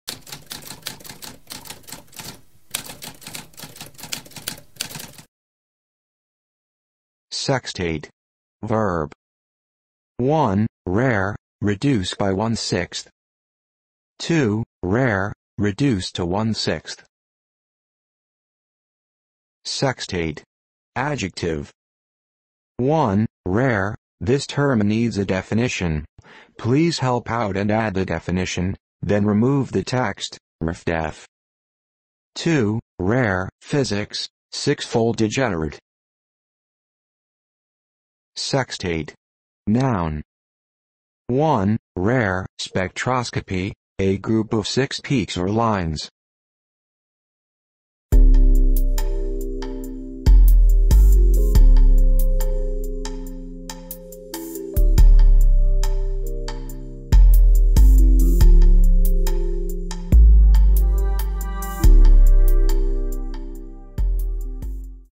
What does sextate mean?
A spoken definition of sextate.

Intro Sound:
Typewriter - Tamskp